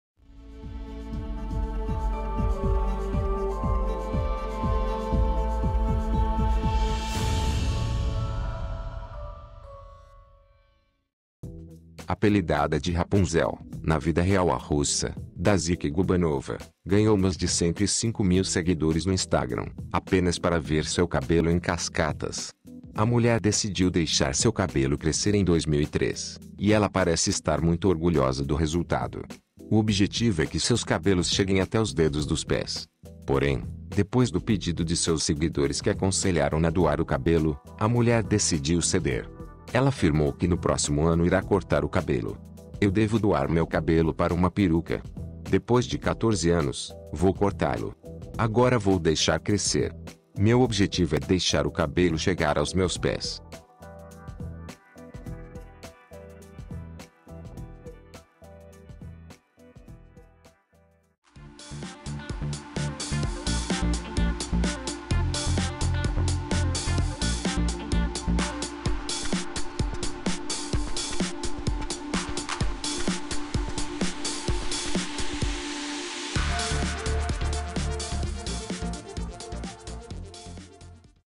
Esta mulher não corta o cabelo a mais de 13 anos, ela é a Rapunzel Russa da vida Real
Apelidada de “Rapunzel”, na vida real a russa, Dashik Gubanova, ganhou mas de 105 mil seguidores no Instagram, apenas para ver seu cabelo em cascatas. A mulher decidiu deixar seu cabelo crescer em 2003, e ela parece estar muito orgulhosa do resultado. O objetivo é que seus cabelos cheguem até os dedos dos pés. Porém, depois do pedido de seus seguidores que aconselharam-na doar o cabelo,  a mulher decidiu ceder. Ela afirmou que no próximo ano irá cortar o cabelo. “Eu devo doar meu cabelo para uma peruca. Depois de 14 anos, vou cortá-lo.  Agora vou deixar crescer. Meu objetivo é deixar o cabelo chegar aos meus pés”.